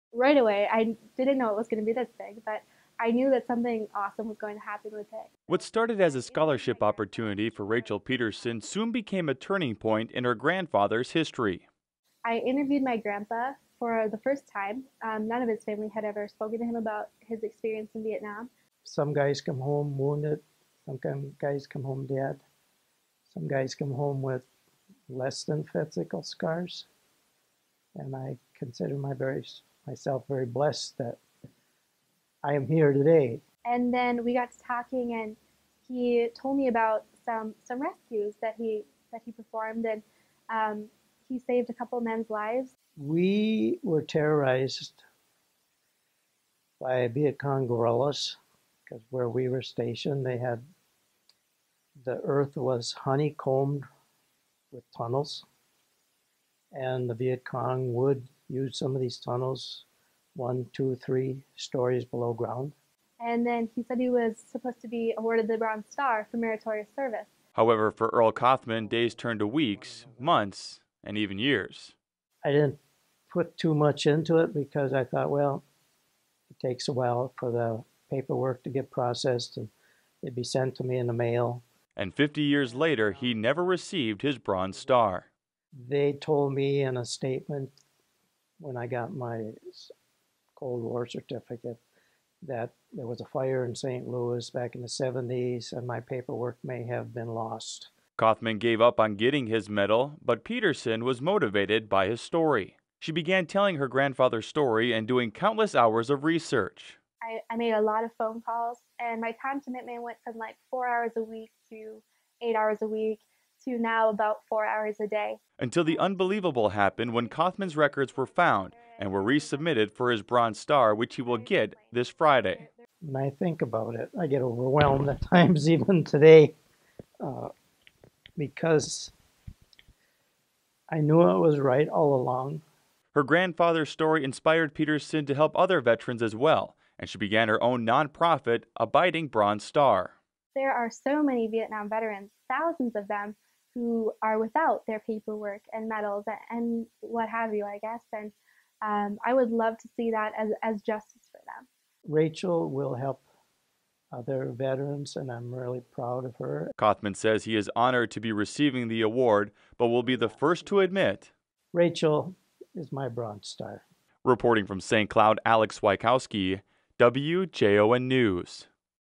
50 Years Later St. Cloud Man To Receive Bronze Star [VIDEO]
A former army sergeant and St. Cloud resident is finally being recognized. What started as a scholarship opportunity for Rachel Petersen soon became a turning point in her grandfathers history.